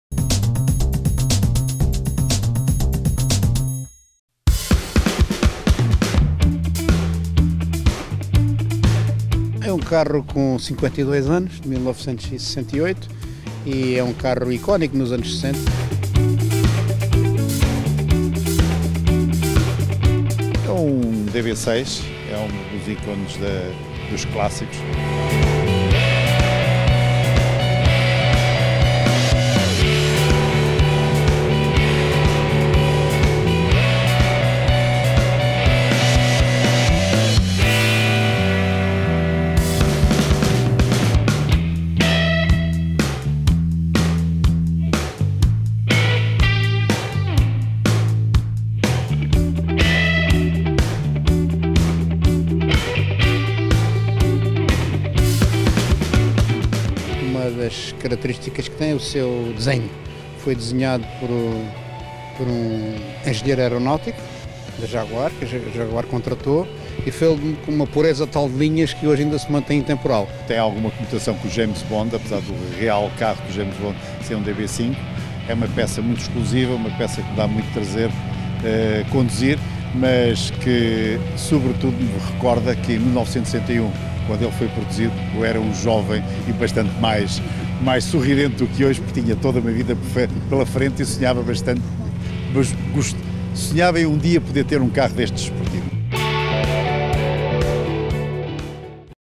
O meu Clássico é melhor que o teu: Aston vs Jaguar
Juntámos dois sócios do ACP Clássicos, um com um Jaguar E, outro com um Aston Martin DB6. No que toca a qual é o melhor, cada um puxou a brasa à sua sardinha. Veja o vídeo e tire as teimas.